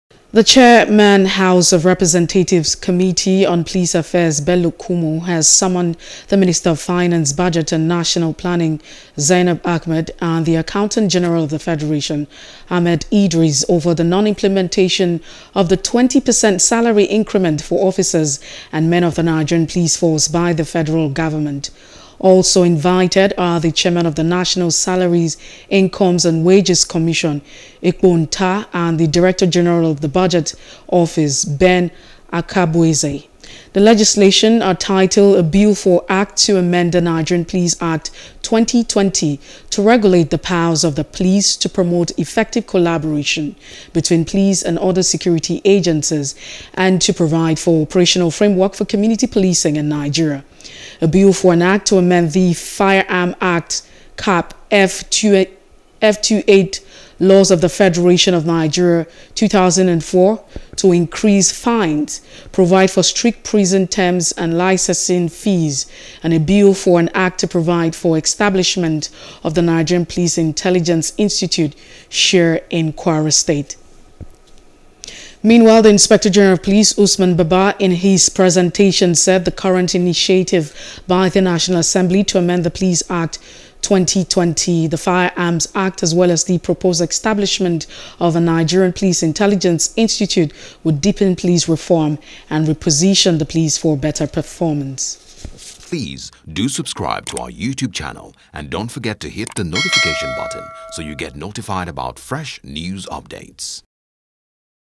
Police Salary Increment: Reps Summon Minister, Accountant General, Others | NEWS
The Chairman House of Representatives’ Committee on Police Affairs Bello Kumo has summoned the Minister of Finance, Budget and National Planning, Zainab Ahmed; and the Accountant-General of the Federation, Ahmed Idris, over the non-implementation of the 20 per cent salary increment for officers and men of the Nigeria Police Force by the Federal Government.